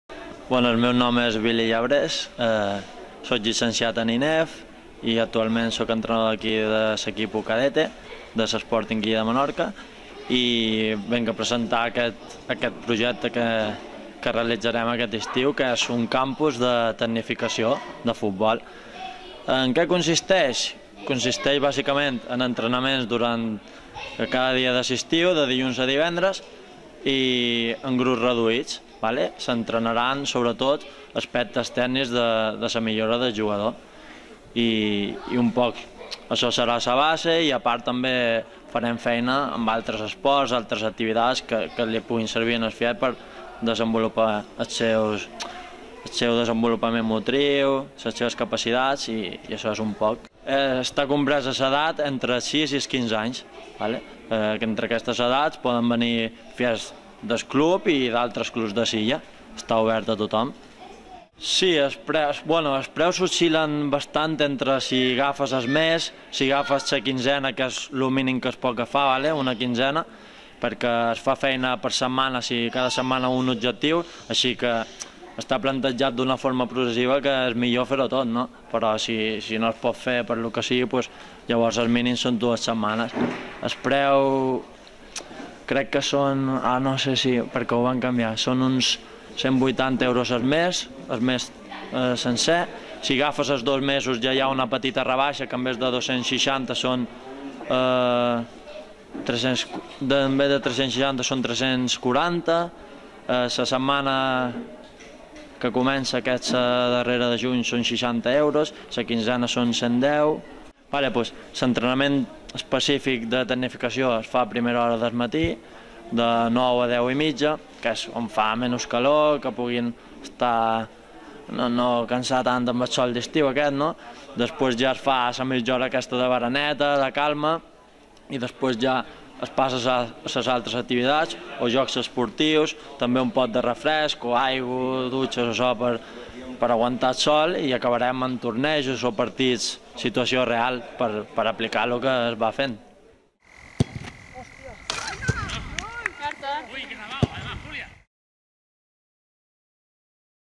Campus Tecnificación SPIM
Presentacion del primer campus de tecnificacion de fútbol organizado por el Sporting en Bintaufa